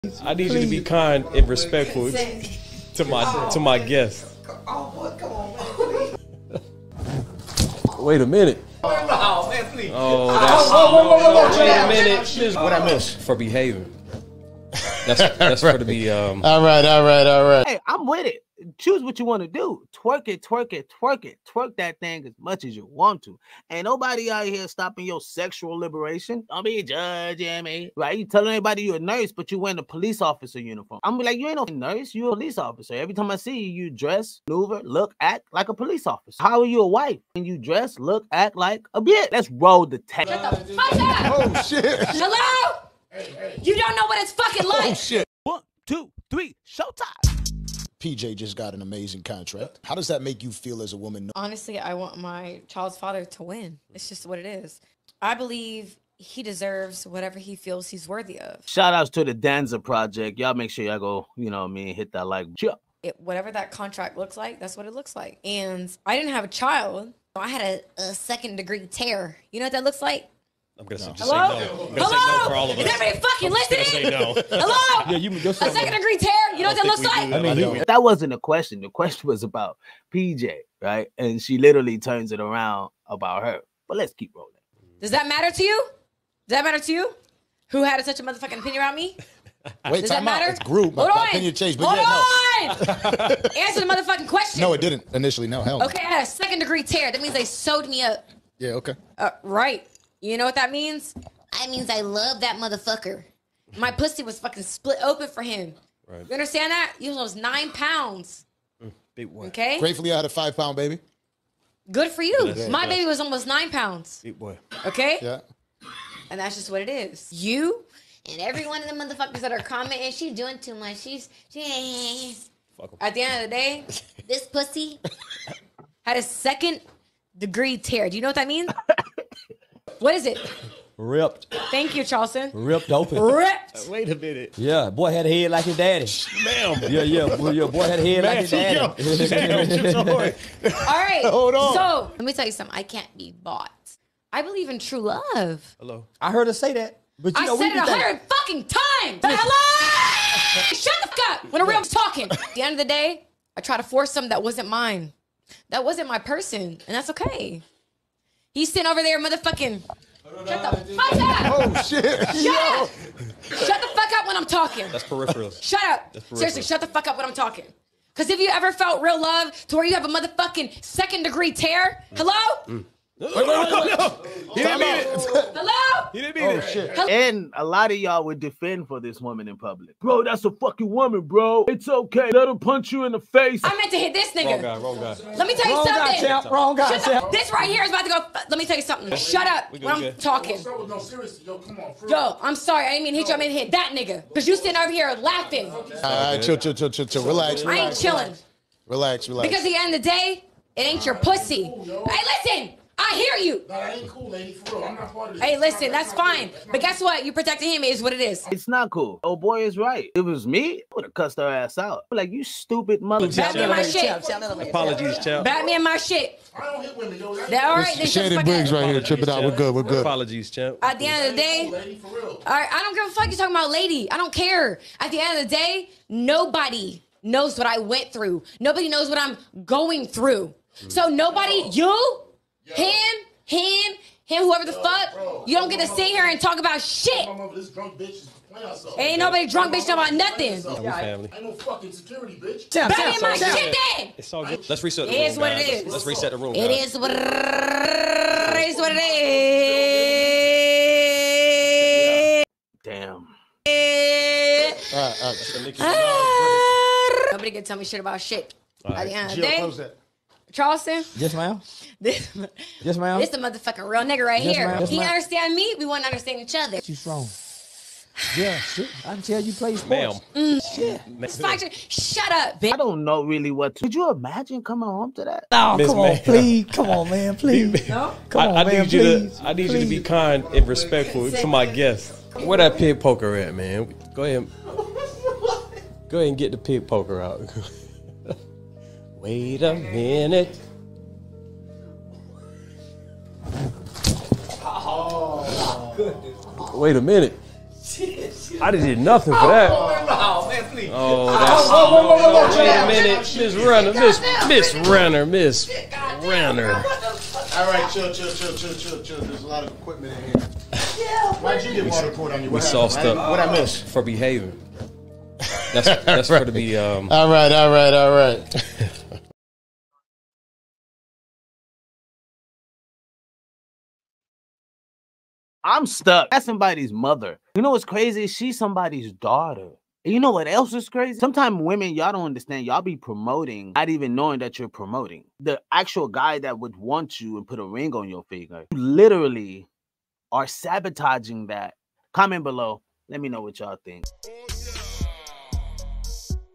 Brittany Renner Goes Crazy about PJ Washington's New Deal...
Brittany Renner GET triggered by PJ Washington's new deal and pours water on Charleston White  @TheDanzaProject  Brittany sitting on Charleston White comes off as wild and drunk...